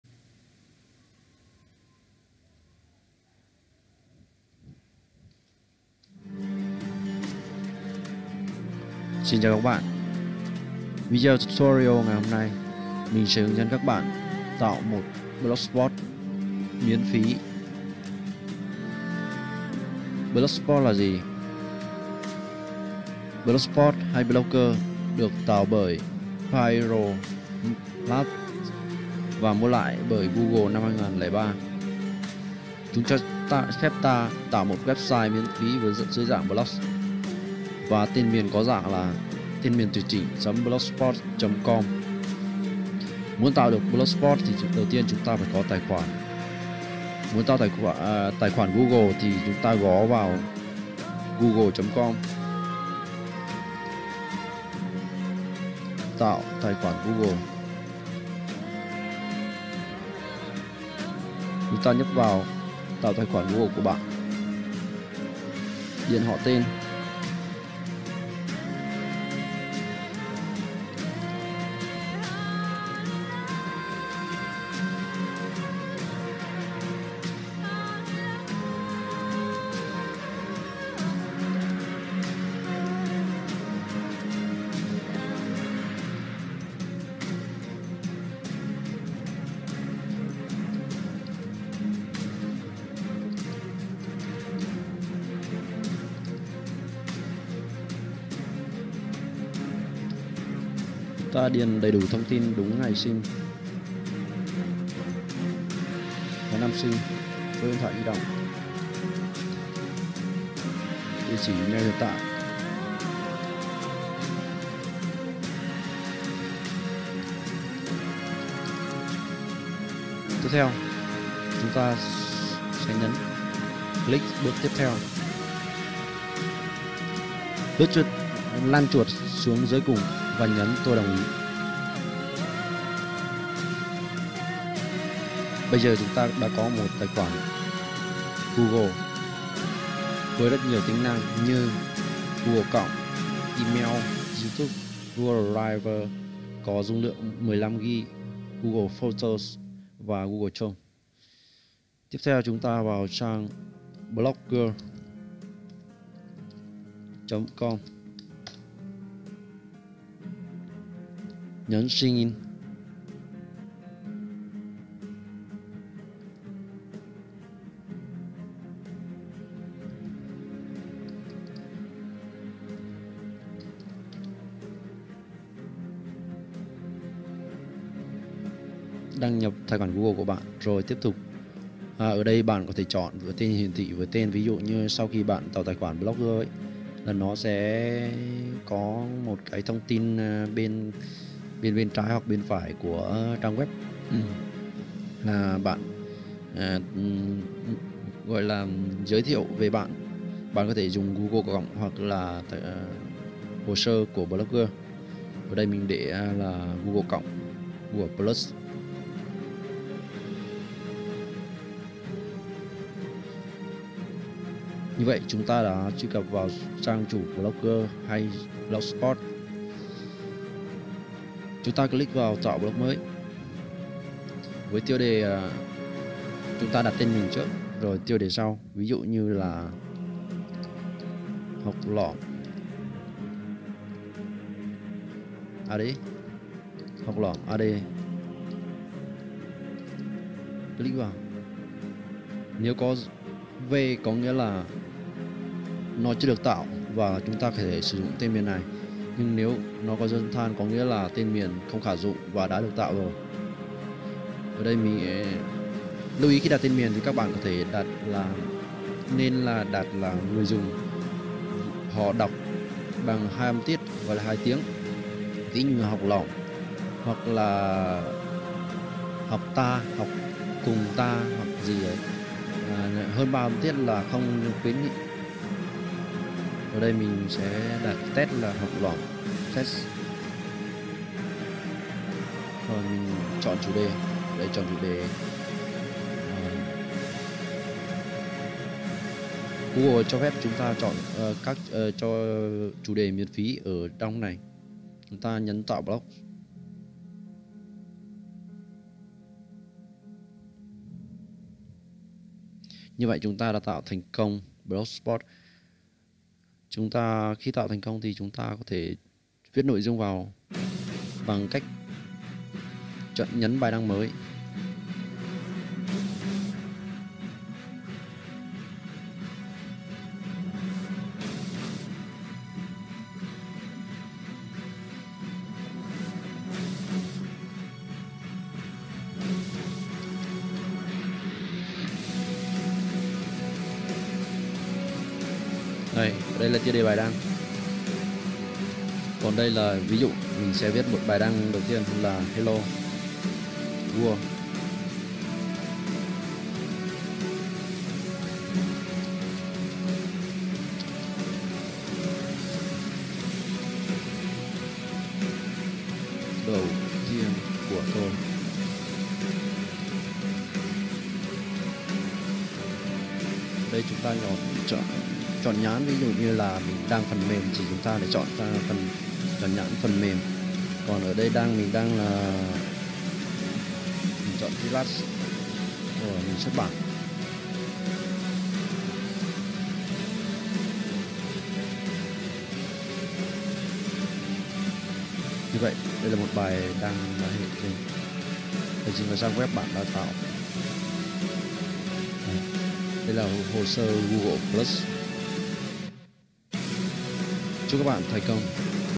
Tạo trang web miễn phí với Blogspot hay Blogger
Blogspot cho phép ta tạo 1 website hay trang web mà không mất bất cứ khoản phí nào !